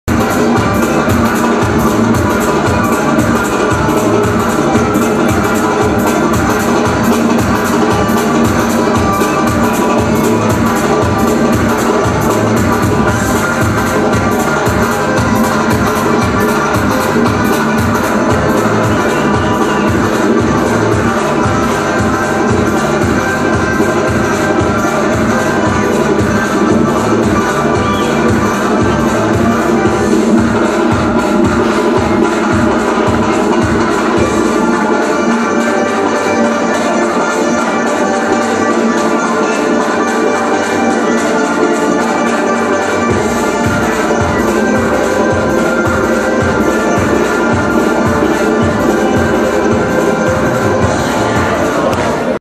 На лакской свадьбе
Шествие молодожёнов к праздничному столу на лакской свадьбе; Махачкала, банкетный зал "Маракеш", 31 октября 2010